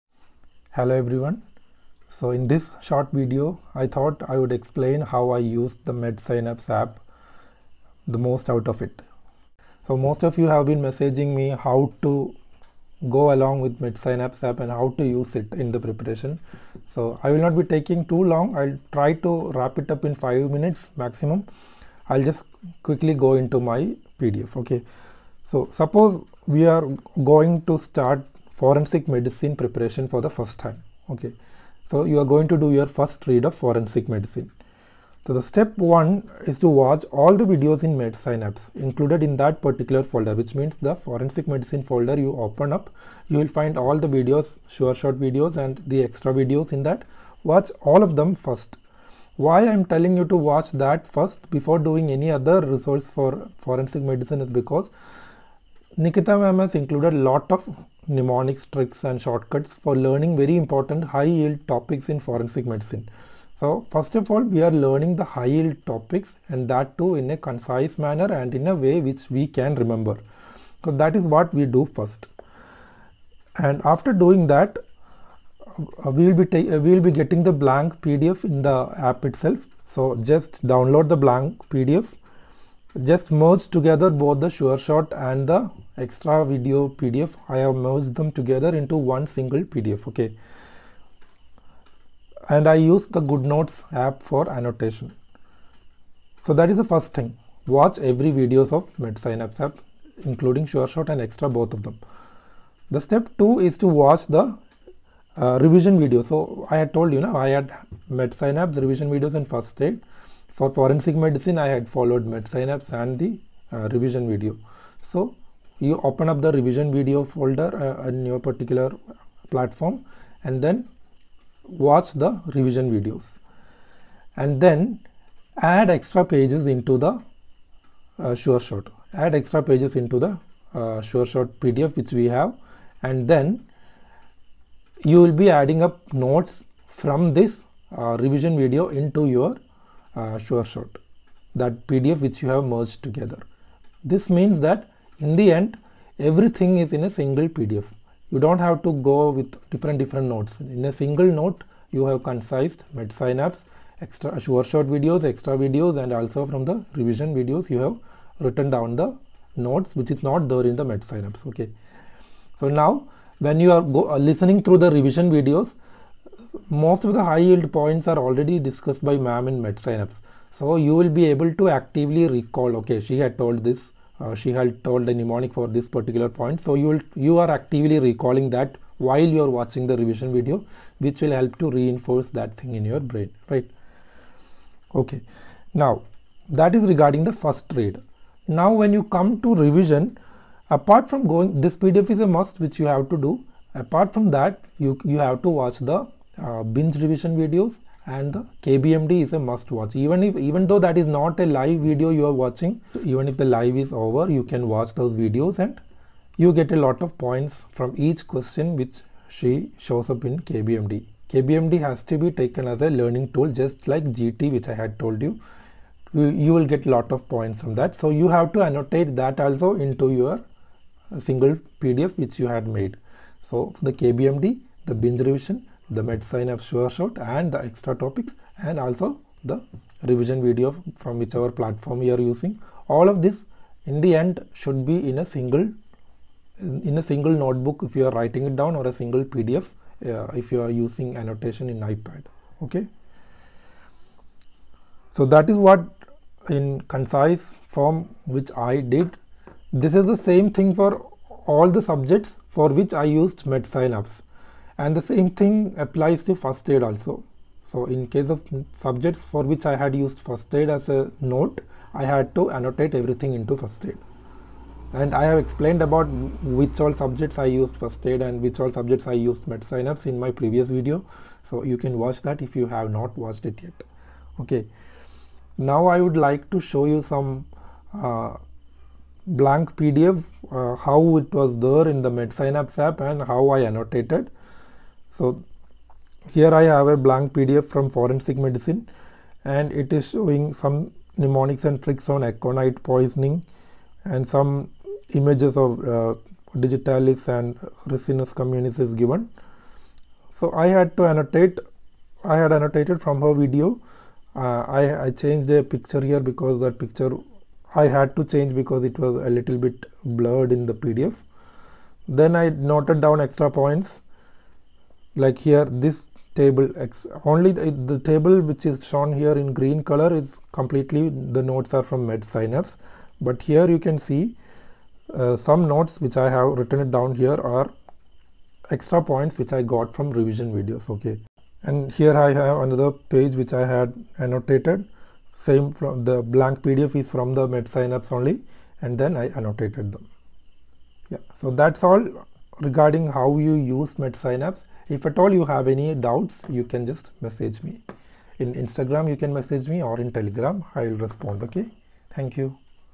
How I used Medsynapse by Dr Nikita effectively for preparation.
In this video I'll explain how I used the medsynapse app by Dr Nikita Nanwani effectively during my entrance preparation.